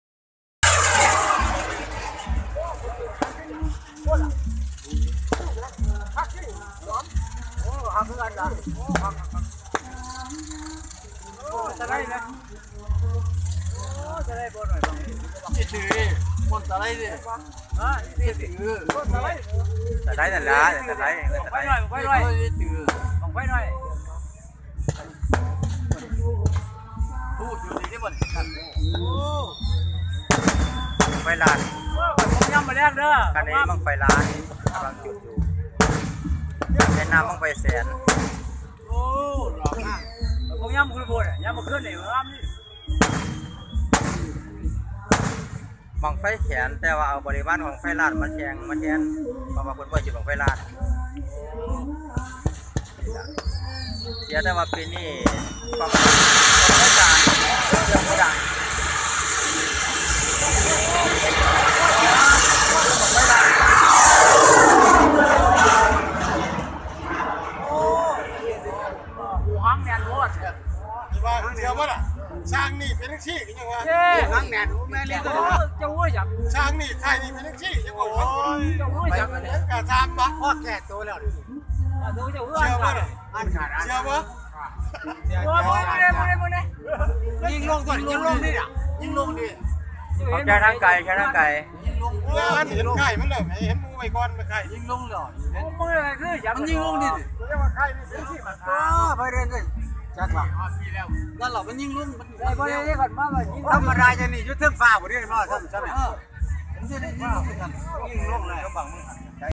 จุดบั้งไฟแสนบริวารไฟล้าน ที่เวทีจุดกลางทุ่งนาบ้านน้ำใส
ปีนี้  (2561) บั้งไฟล้านห้ามจุดที่สนามจุดบั้งไฟบ้านน้ำใส  บริวาร หรือหางเครื่องของบั้งไฟล้านจึงมาประดับสร้างสีสันให้กับบั้งไฟแสน  ท่านใดที่ไม่ได้มาดูชม เชิญทัศนาได้ขอรับ